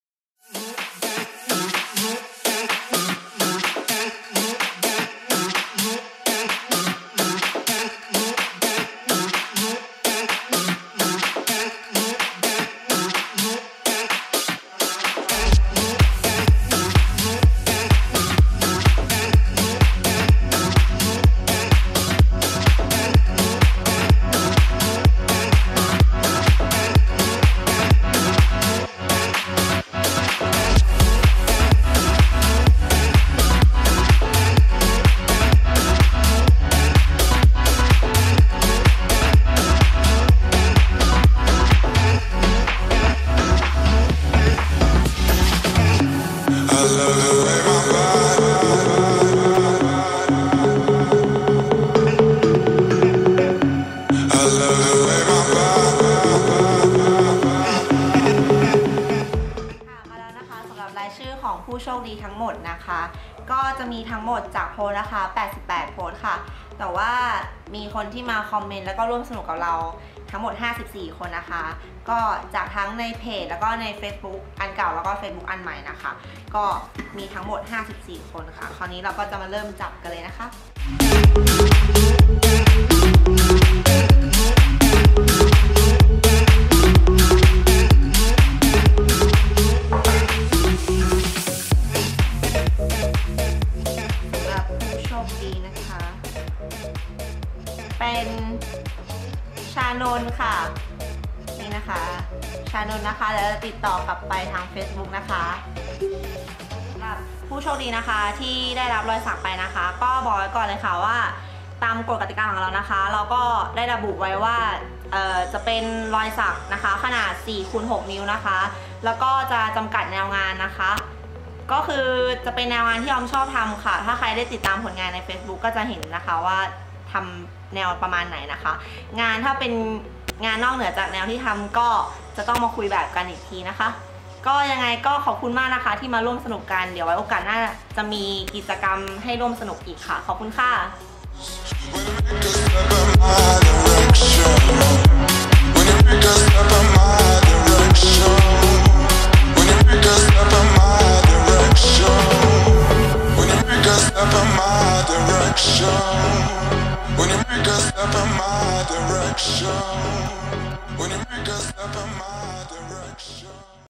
ZERO TATTOO : Event ep1
มาแล้วววว.
สำหรับรายชื่อผู้โชคดีที่มาร่วมสนุกกับกิจกรรม โพสและแชร์ " ZERO TATTOO "
แล้วทางเราจะติดต่อกลับไปนะค้าา
ขอบคุณทุกคนที่มาร่วมสนุก ไว้มาร่วมกิจกรรมกันใหม่นะค้าาา